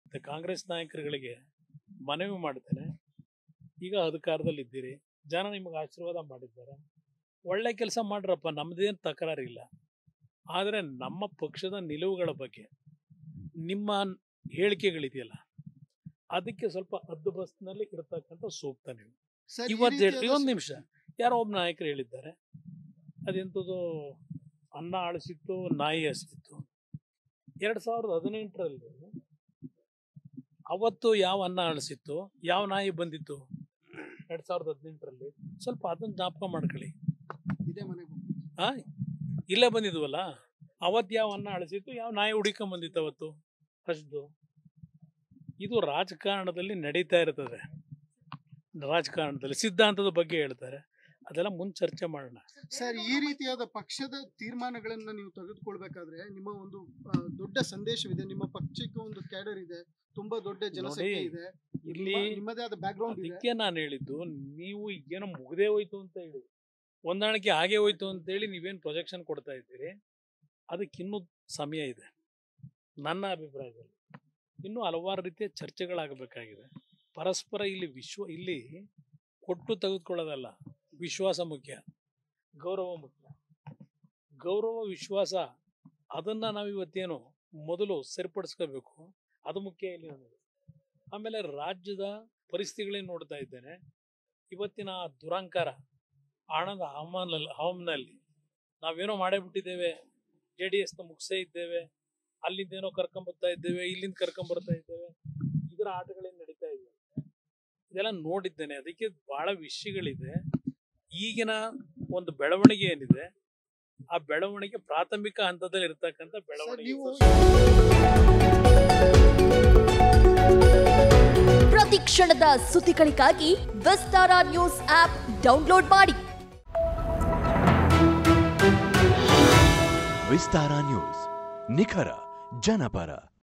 ಕಾಂಗ್ರೆಸ್ ನಾಯಕರ ವಿರುದ್ಧ HDK ಗರಂ! | HD Kumaraswamy Slams Congress On Alliance | Vistara News
ಅನ್ನ ಹಳಸಿತ್ತು ನಾಯಿ ಹಸಿದಿತ್ತು ಎನ್ನುವ ಕಾಂಗ್ರೆಸ್ ನಾಯಕರ ಟೀಕೆಗೆ ಮಾಜಿ ಸಿಎಂ ಎಚ್.ಡಿ.ಕುಮಾರಸ್ವಾಮಿ ತಿರುಗೇಟು ನೀಡಿದರು.